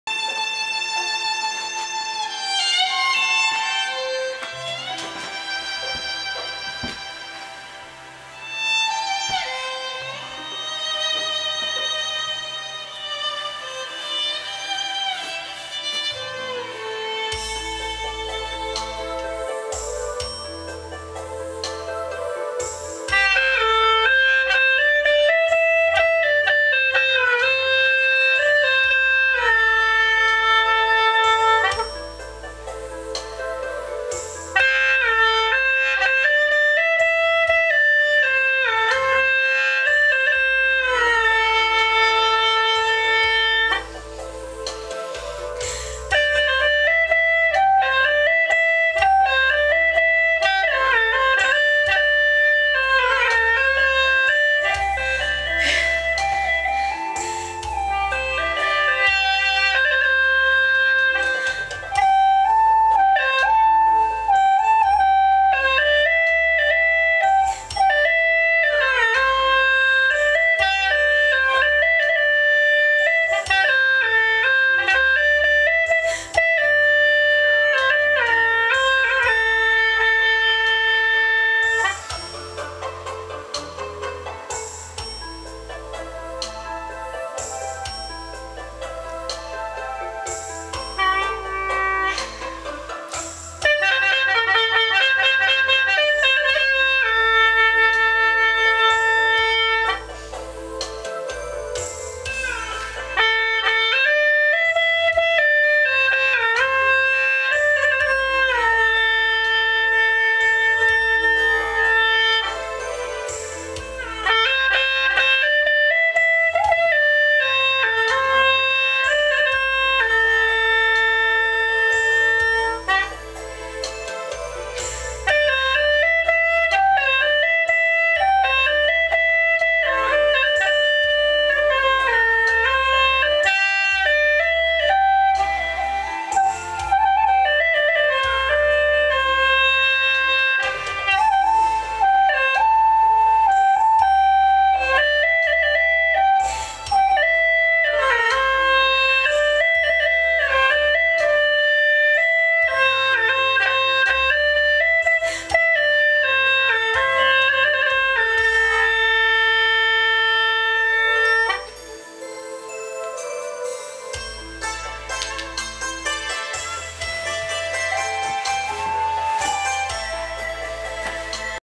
That song. Resurrected.
I'd like to give a shout out to all of my SIT-ers who walked the streets of Dali, Lijiang and every other tourist attraction in northern Yunnan---and random dining halls--and were blessed with this beautiful & omnipresent music.